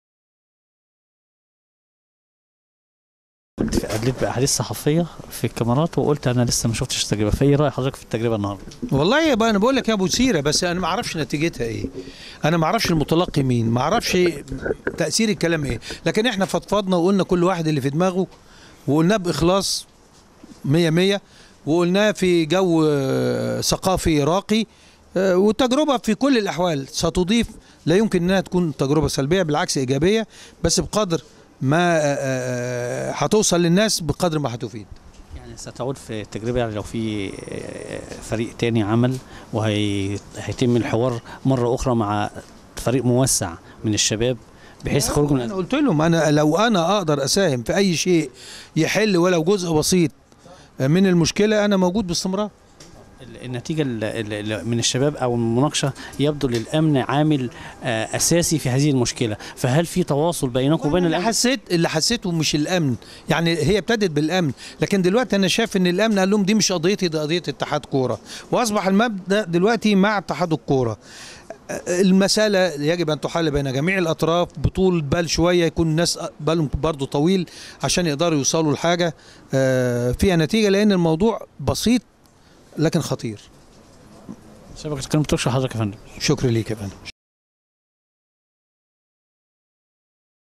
عدلي القيعي في حوار خاص لشبكة يقين الإخبارية
حصري للشبكة في ندوة الحوار مع ألتراس الأهدلي في مكتبه أ